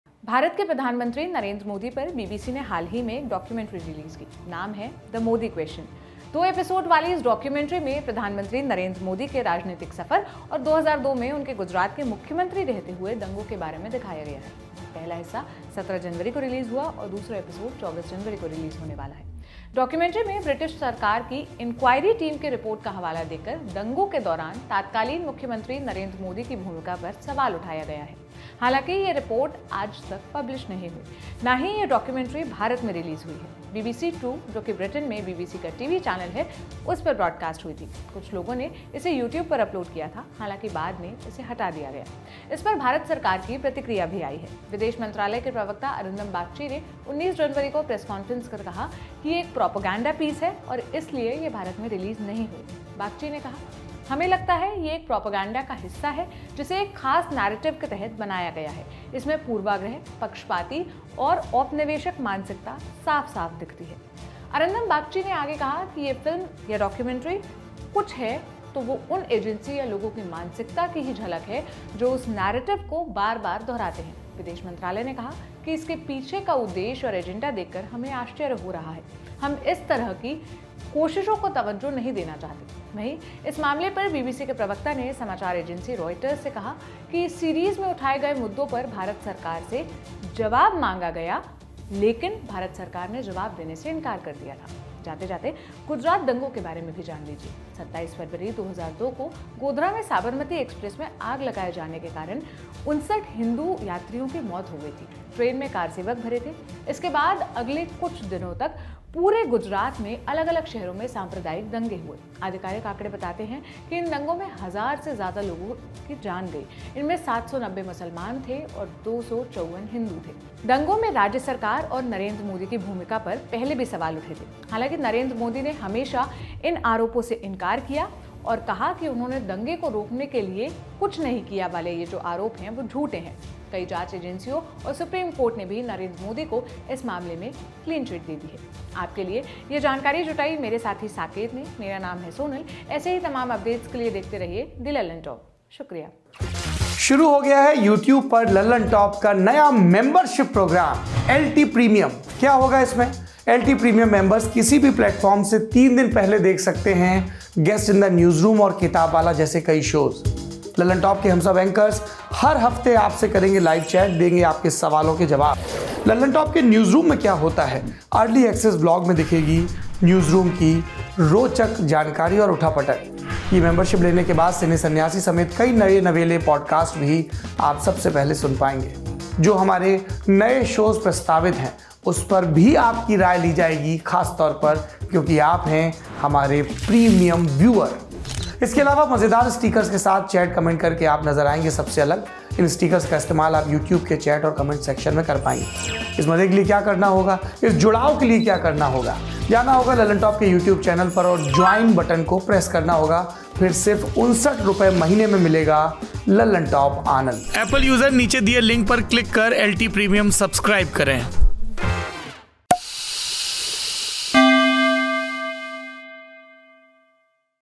BBC Documentary में गुजरात दंगों पर क्या है जिस पर विवाद हो रहा है? | Gujarat Riots | PM Modi
BBC Documentary On PM Modi : BBC made a series on Prime Minister Narendra Modi and the 2002 Gujarat riots  called "India: The Modi Question". Indian government reacted on it and called it a "propaganda piece designed to push a discredited narrative" that shouldn't be "dignified" with a response. MEA Spokesperson Arindam Bagchi did a press conference and shared Indian Government's stand on it. Watch the video to know more about it.